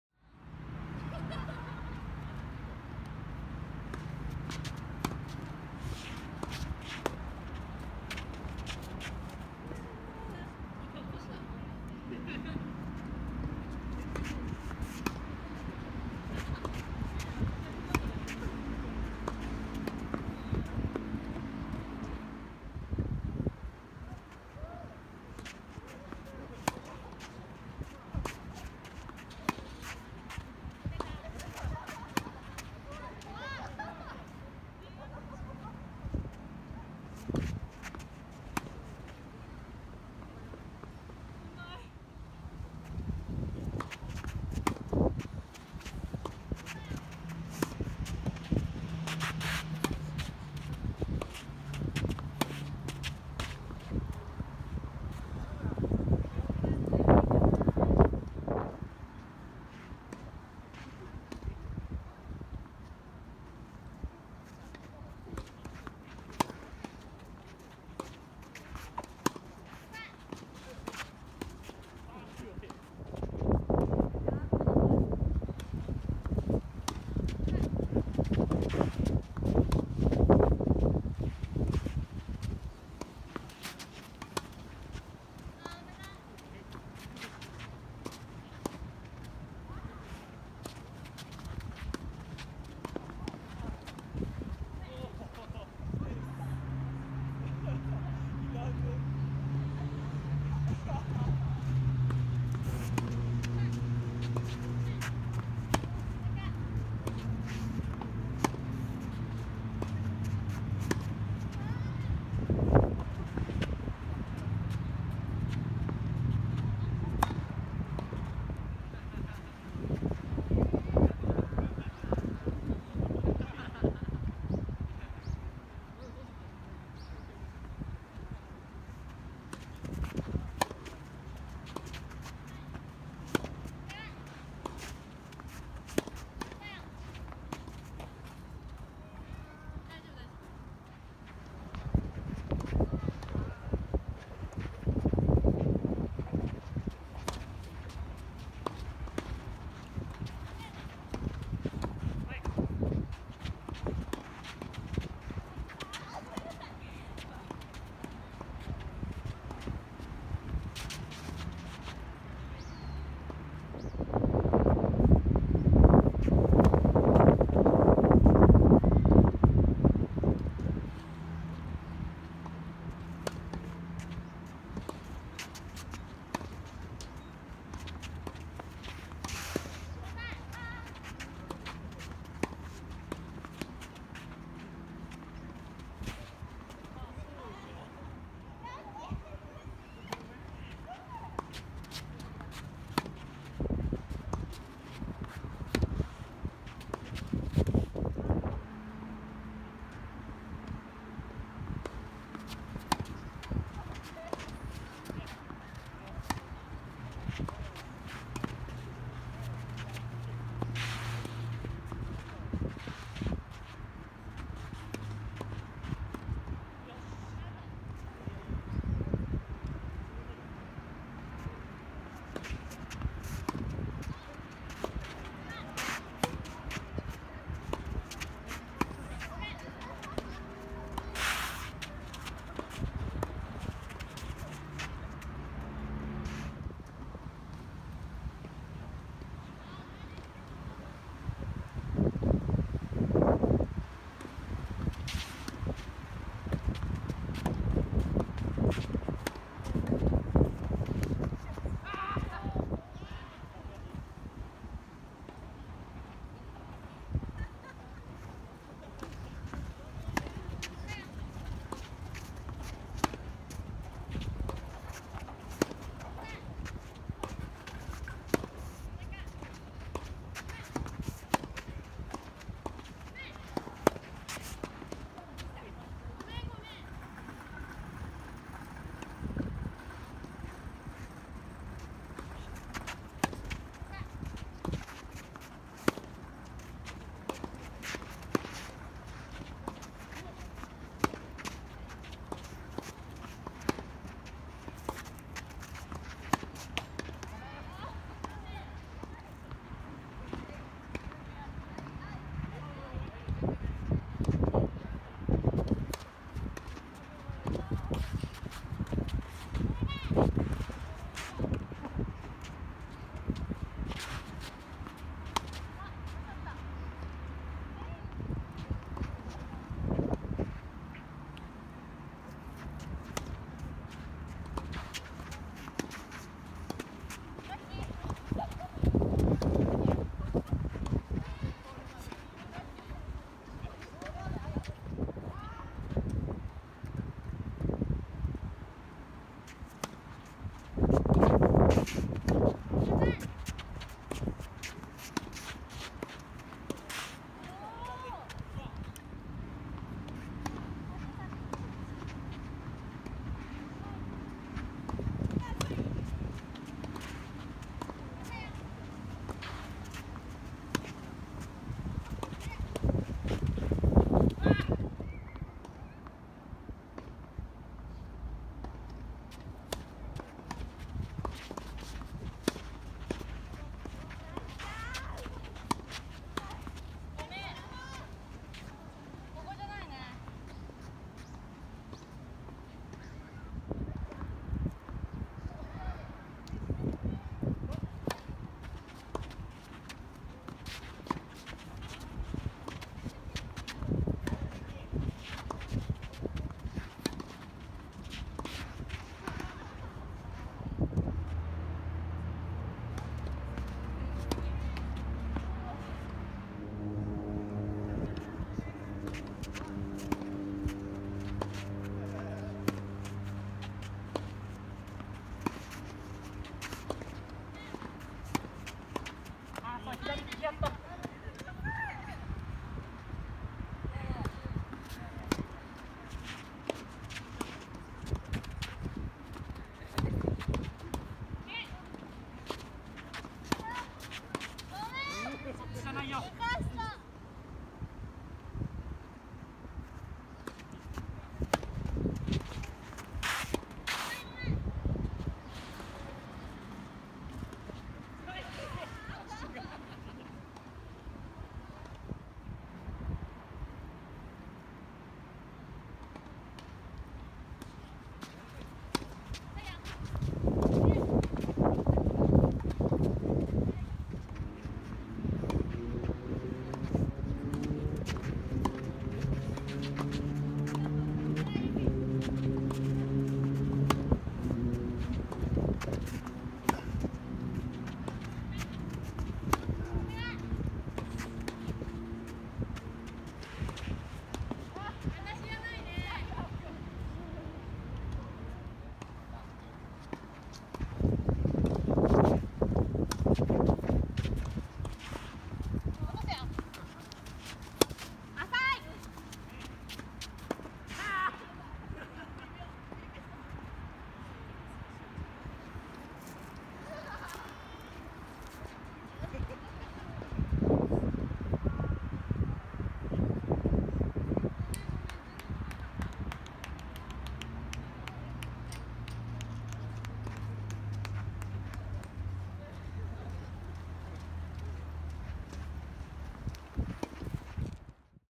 【第1回ビラージュミックス/決勝】萬福健太郎/山口あやみ VS 西上尚志/西上捺美
2021/2/27 第1回ビラージュミックス

◯　萬福健太郎/山口あやみ VS 西上尚志/西上捺美
6-4